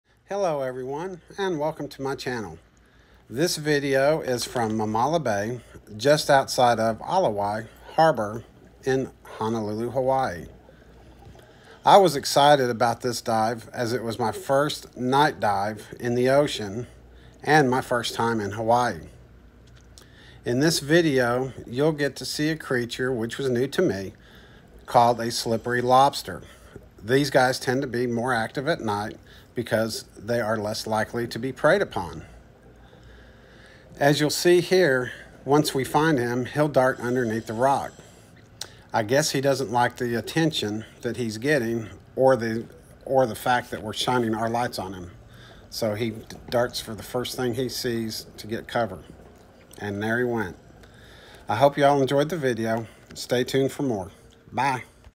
Night dive in Honolulu Hawaii
During this dive we saw a slippery lobster.